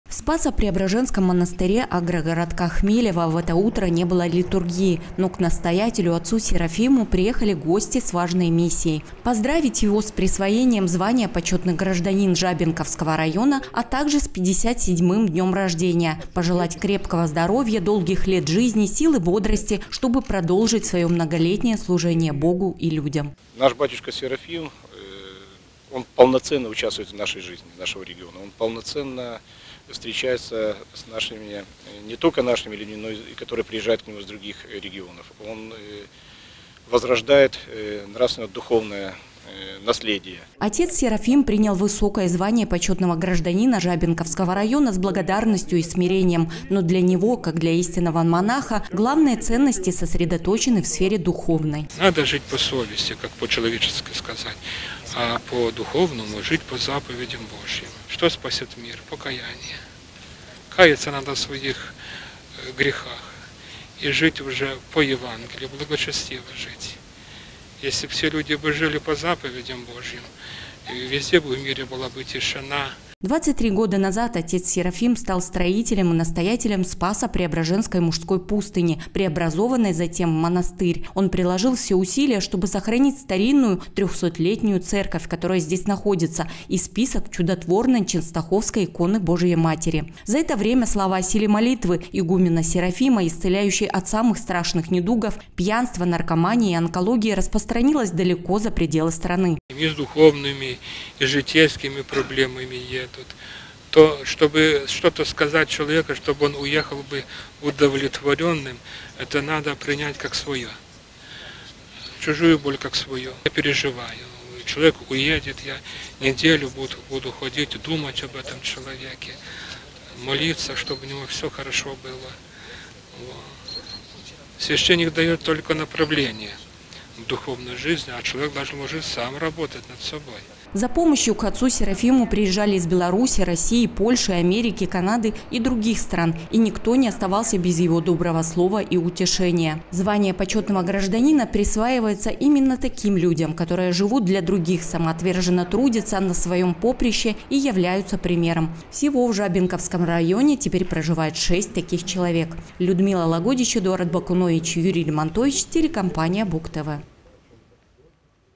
2022-11-02 г. Брест. Присвоение звания «Почетный гражданин». Новости на Буг-ТВ.
Присвоение звания «Почетный гражданин» наместнику Спасо-Преображенского монастыря в агрогородке Хмелево Серафиму Петручику.
Почётный гражданин - это звание, которое присваивается за особые заслуги и символизирует исключительную форму выражения признательности и благодарности общества. В Жабинковском районе до настоящего момента его были удостоены 5 человек. Шестым по предложению Жабинковского райисполкома и при единодушной поддержке районного Совета депутатов стал наместник Спасо-Преображенского монастыря в агрогородкеХмелево Серафим Петручик.  В день своего рождения, человек, который всю свою жизнь положил на служение Богу и людям принимал двойные поздравления.